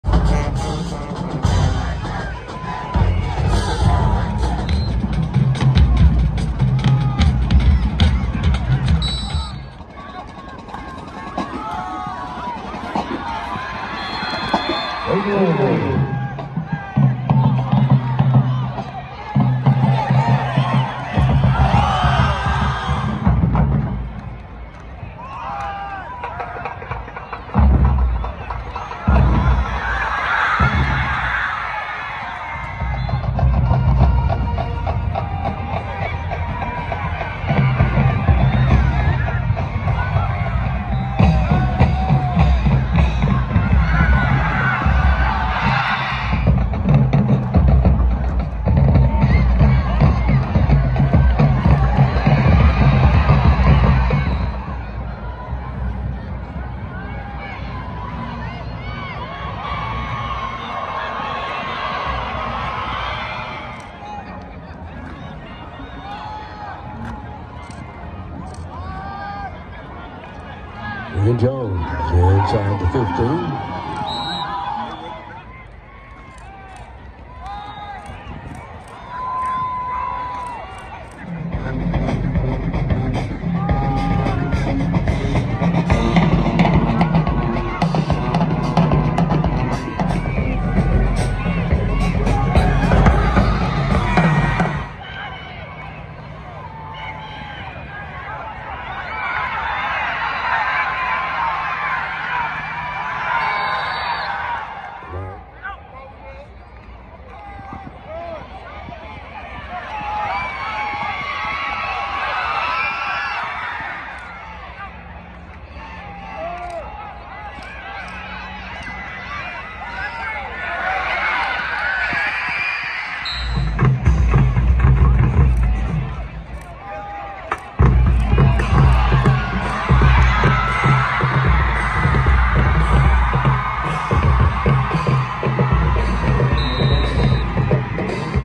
TXHSFB Pinkston vs. Carter
Carter picks up a big district win after defeating Pinkston, 32-7‼️